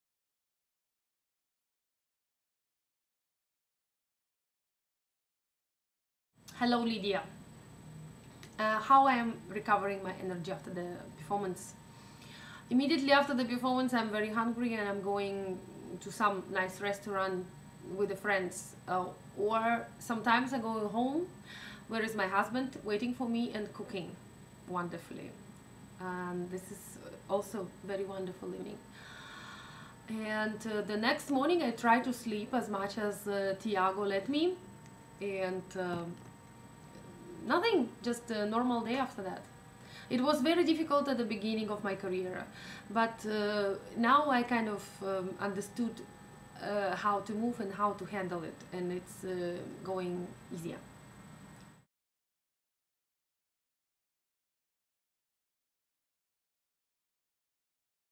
Ask Anna: Recovery After Performance
Question: "How do you recover after a concert? What is the secret so fresh, beautiful, and energized?" - Lidiya Kolesnik, United States

Ask Anna: The Official Video Blog of Anna Netrebko